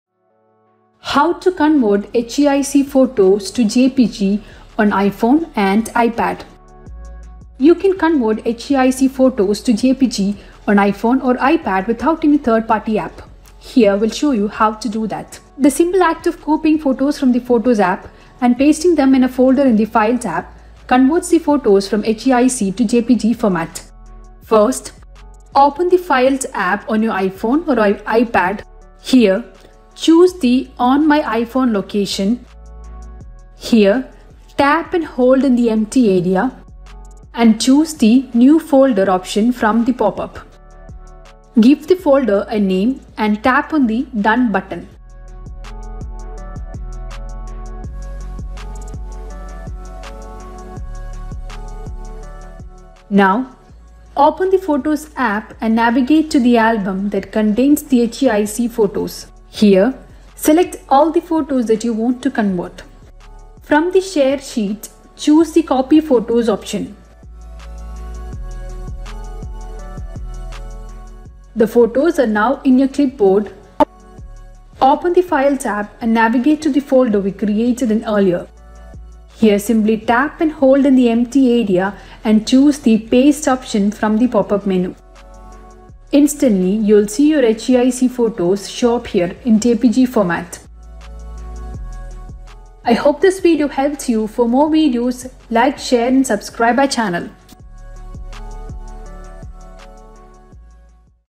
How to Convert HEIC Photos to JPG on iPhone
You can Convert HEIC Photos to JPG format on iPhone or iPad without any third-party app for free. Here we will show you how to do that.
Copying photos from the Photos app and pasting them into a folder in the Files app converts the images from HEIC to JPG format.
1. open the Files app on your iPhone or iPad.
2. choose the “On My iPhone” location
3. tap and hold in the empty area, and choose the “New Folder” option from the popup.
4. Give the folder a name, and tap on the “Done” button.
5. open the Photos app and navigate to the album that contains the HEIC photos.
6. select all the photos that you want to convert.
7. Choose the “Copy Photos” option from the Share sheet.
8. Open the Files app and navigate to the folder we created earlier.
9. simply tap and hold in the empty area and choose the “Paste” option from the popup menu.

Instantly, you’ll see your HEIC photos show up here in JPG format without losing quality. How to mass convert heic to jpg